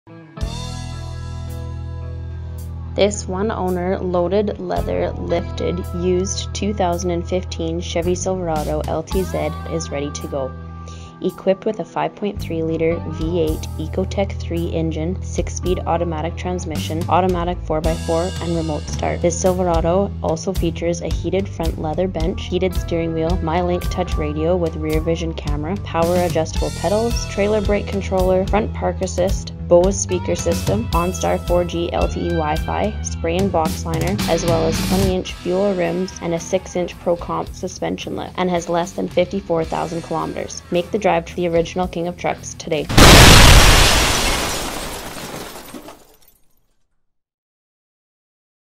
45908 2015 Chevrolet Silverado LTZ for sale in Saskatchewan
This one owner, loaded, leather, lifted, used 2015 Chevy Silverado LTZ 1500 for sale in Saskatchewan on our Mainline Motors GMC, Buick, and Chevrolet lot is ready to go! Equipped with a 5.3L V8 Ecotec3 engine, 6-speed automatic transmission, auto 4x4, and remote start! This Silverado also features a heated front leather bench, heated steering wheel, MyLink touch radio with rear vision camera, power adjustable pedals, trailer brake controller, front park assist, Bose speaker system, OnStar 4G LTE wi-fi, spray-in box liner, as well as 20" Fuel rims, a 6" ProComp suspension lift, and less than 54,000 km! Make the drive to Rosetown Mainline Motors, the ORIGINAL King Of Trucks, today!